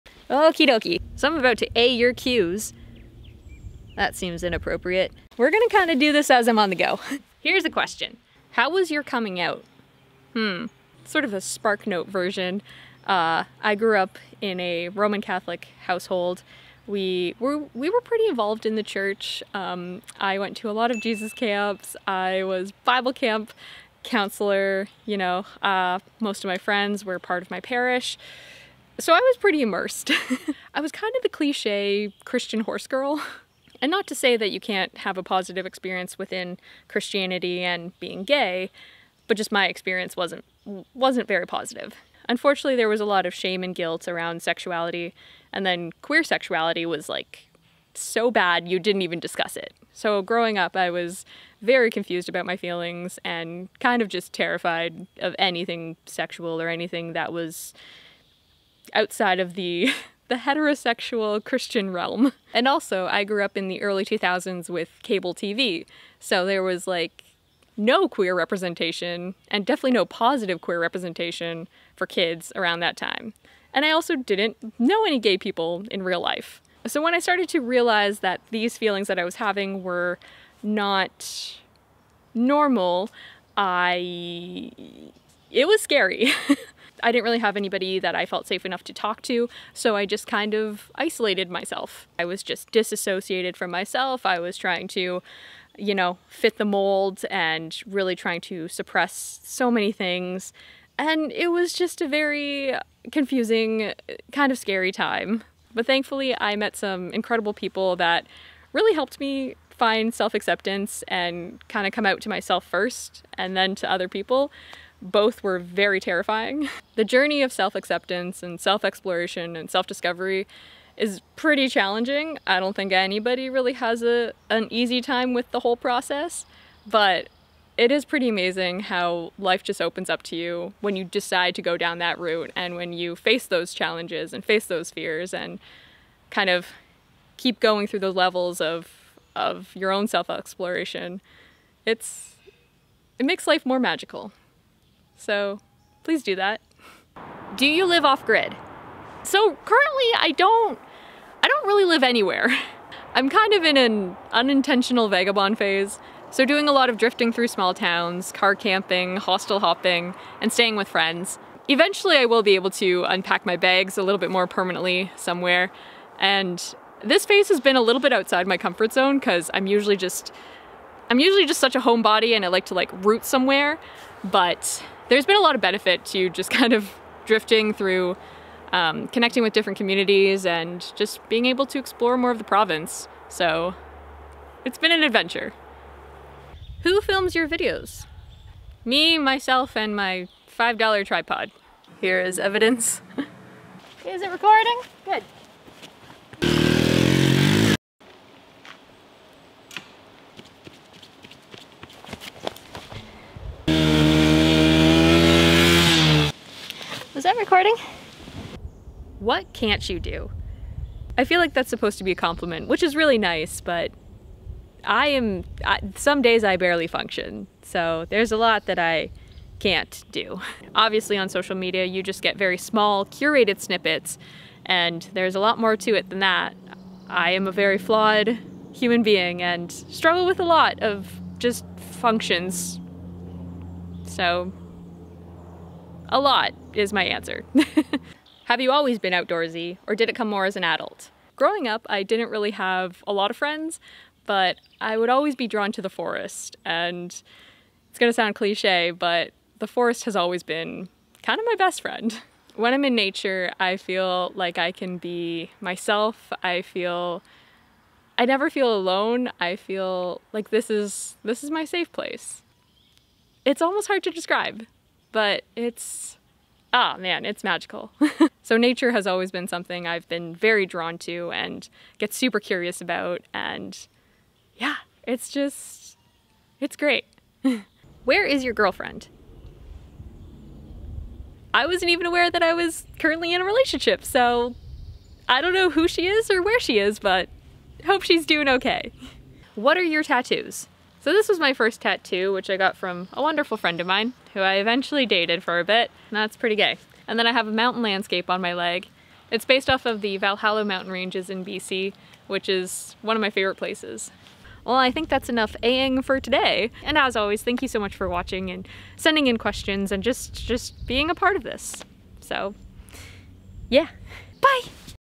Jesus Camps, Coming Out, Partner? │ Q & A
Questions:
0:08 How was your coming out?
2:25 Do you live off-grid?
3:05 Who films your videos? (and edits)
3:28 What can't you do?
3:57 Were you always outdoorsy or did it come more as an adult?
4:37 Where is your girlfriend?
4:49 What are your tattoos?